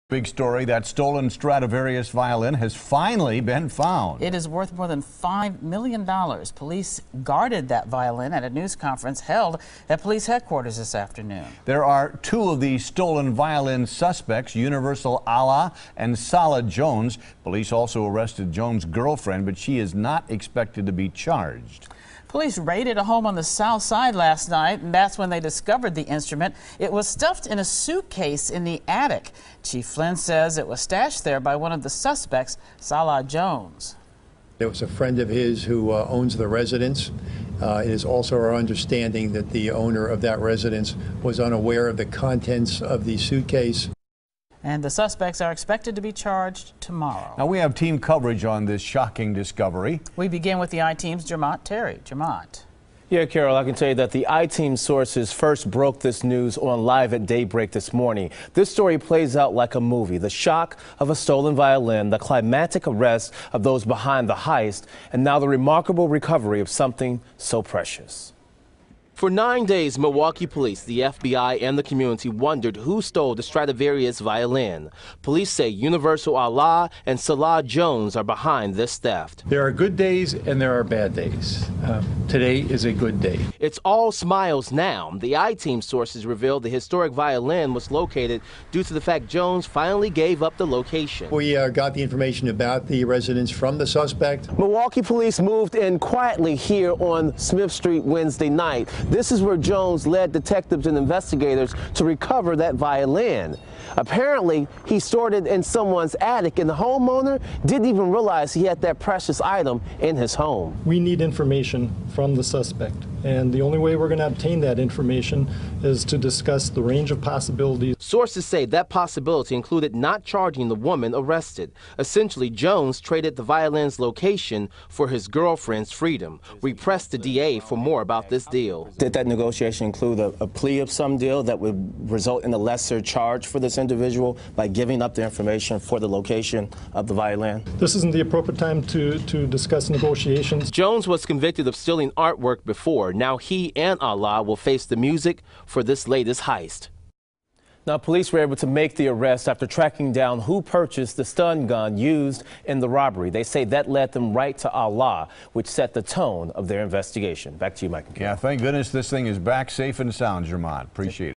Stolen violin discovered in Bay View home
Police found the stolen Stradivarius violin in a suitcase, in the attic of a Bay View home. Neighbors, and apparently even the owner, didn't realize what was upstairs. The I-Team's Jermont Terry has more.